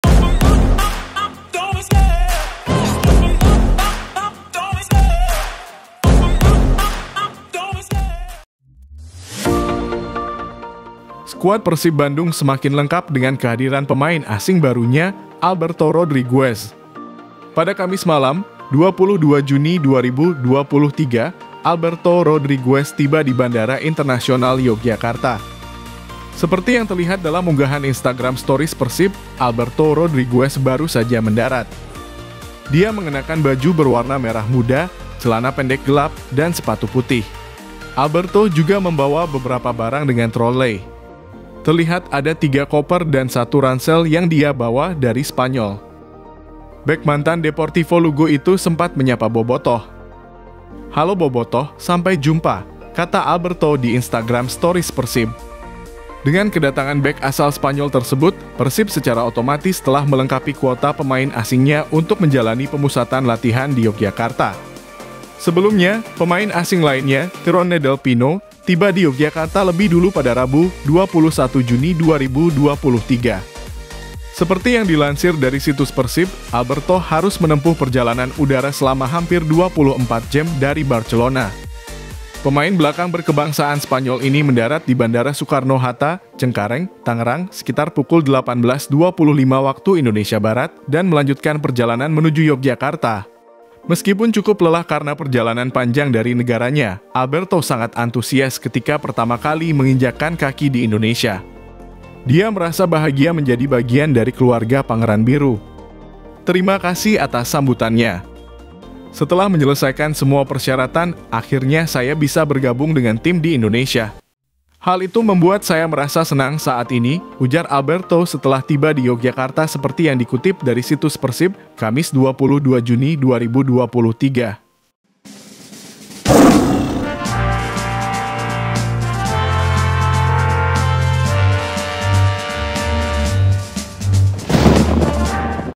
🔴 BREAKING NEWS - TIBA DI YOGYA, INI KOMENTAR MENGEJUTKAN ALBERTO RODRIGUEZ UNTUK BOBOTOH
✍️ ALHAMDULIILLAH DAISUKE DAN ALBERTO TIBA !!  TOP 2 Kabar PERSIB Terkini & TERUPDATE Hari Ini, RABU 21 Juni 2023

REFERENSI BERITA:

okebola,CNN,DETIKSPORT,OFFICIALPERSIB,OKEZONE,PERSIBCOID,BOLASPORT,TRIBUNJABAR,BOLA,GOAL,AYOBANDUNG
Republika, Simamaung